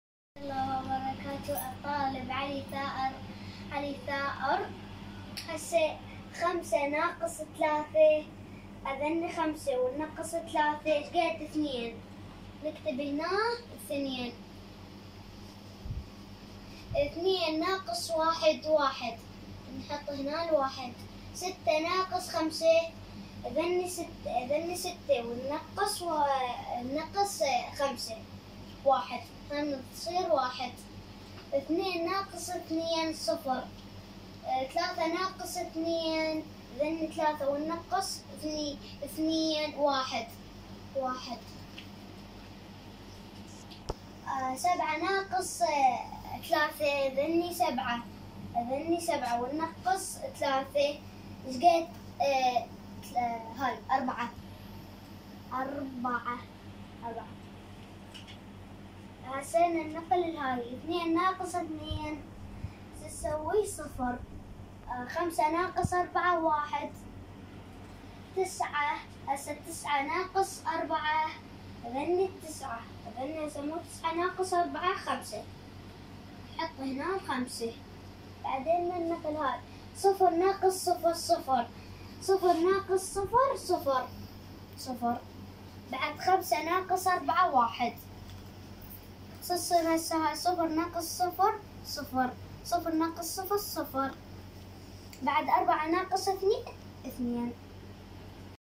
طلابنه المتميزين بالدورات التعليميه الطالب المتفوق علي ثائر الصف الثاني رياضيات عمليه الطرح
‏فيديو من ام عمار